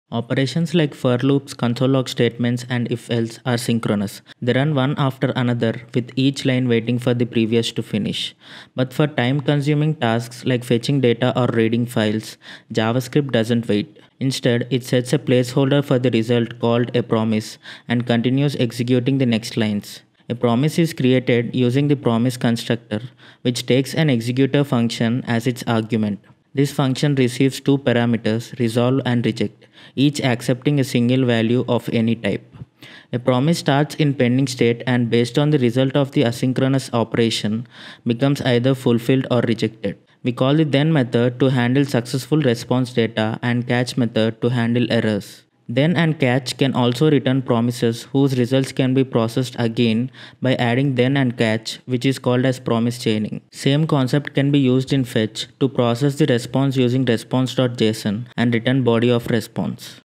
JavaScript Promises Explained Simply 🔥
Struggling to understand Promises in JavaScript? In this video, we break down what Promises are, how they work, and why they’re essential for handling asynchronous operations like API calls and file reading.

You’ll learn:
✅ What is a Promise in JavaScript
✅ How to create and use Promises
✅ Understanding then(), catch(), and Promise chaining
✅ Real-world example using fetch() and response.json()